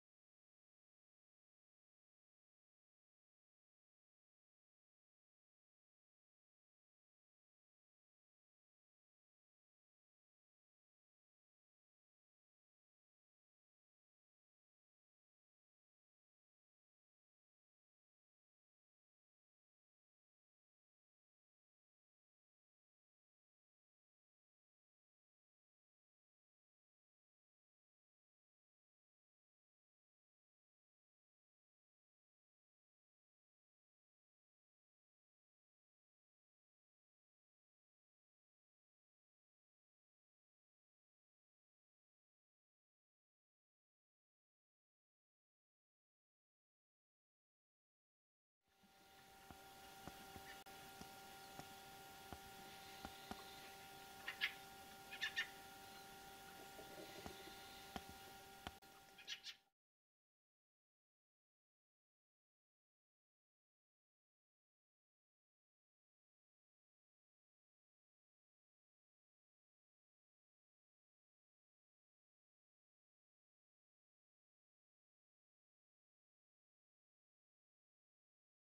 Wacholderdrosseln in Schielehen
Wacholderdrosseln und Amseln fressen Äpfel in Schielehen am 6. Januar 2021.